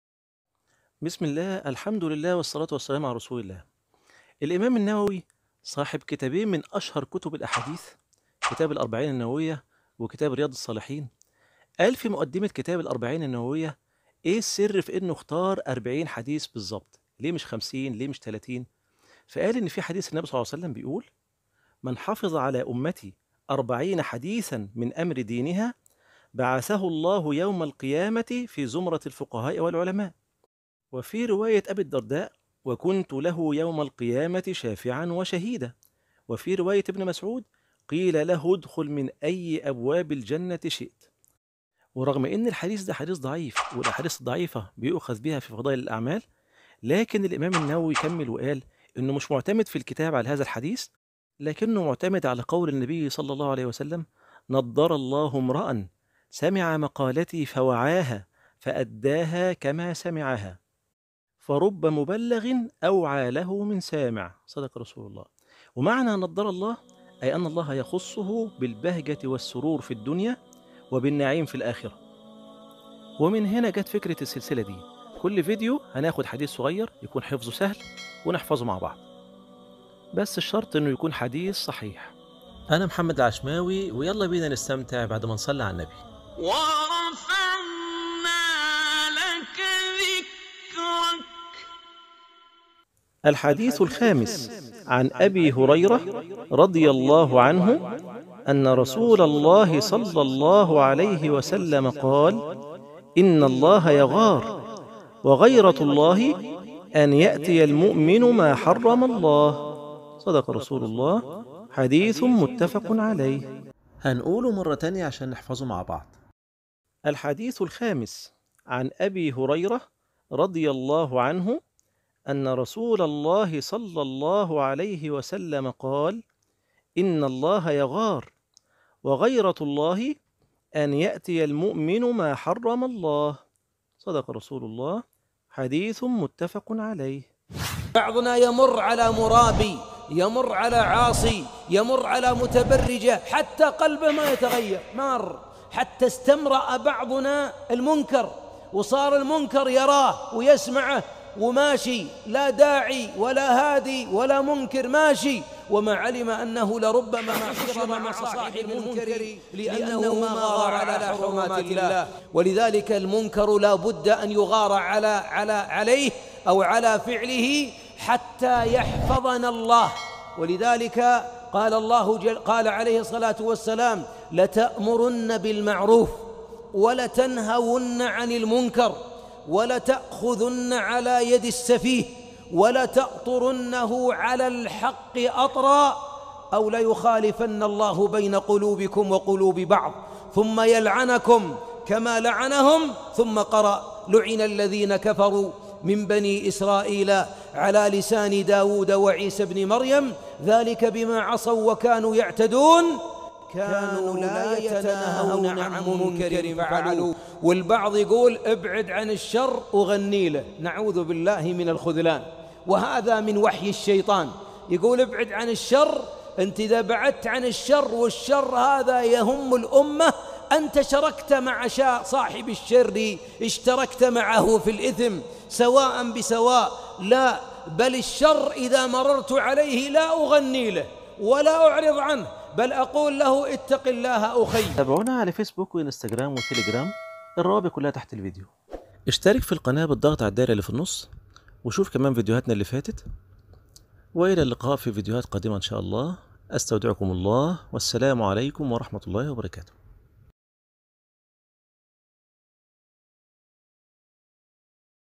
5️⃣ "غَيرَةُ الله" هل يغار الله؟ 🤔 و علي ماذا يغار! خواطر الشيخ سعد العتيق🌹
اسهل طريقة لحفظ وفهم الحديث 👌 سلسلة حديث صحيح سهل الحفظ ❤️
فمن صفات الله تعالى الغيرة كما ثبت في هذا الحديث فيجب أن نؤمن بهذه الصفة لله عز وجل ولكنها ليست كغَيرَة المخلوقين.

فهو سبحانه {ليس كمثله شيء وهو السميع البصير} الشورى 11 و {هل تعلم له سميا} مريم 65 و {ولم يكن له كفوا أحد} الإخلاص 4
ففي هذا الحديث ثبت أن الله عز وجل يغار وغيرته أن يأتي المؤمن ما حرم الله عليه، أي أن الله عز وجل يغار أن يأتي عبده المؤمن المحرمات ويرتكبها، وخص الله عز وجل المؤمن في غيرته لأن انتهاك المحرمات من المؤمن أعظم عند الله من انتهاكها من غير المؤمنين، فليس بعد الكفر ذنب، وأيضا المؤمن أعلى درجة من المسلم فارتكاب المحرمات من المؤمن أعظم عند الله من ارتكابها من قبل المسلم،
فاحذروا إخوتي وأخواتي من ارتكاب المحرمات، فالله عز وجل يغار أن يقع المؤمنين فيما حرمه الله عز وجل،
فمن صفات الله عز وجل الغيرة على اتيان المحرمات من قبل عباده المؤمنين، وكما قلنا صفات الله عز وجل نؤمن بها من غير تأويل ولا تكييف ولا تمثيل ولا تحريف ولا تعطيل، فالله عز وجل ليس كمثله شيء .. ولا نعلم له سميا .. وليس له كفواً أحد,
مع شرح الغيرة علي محارم الله للشيخ سعد العتيق و وجوب الأمر بالمعروف و النهي عن المنكر و الأخذ علي يدي الظالم حتي يرجع عن ظلمه.

* سلسلة حديث صحيح سهل الحفظ *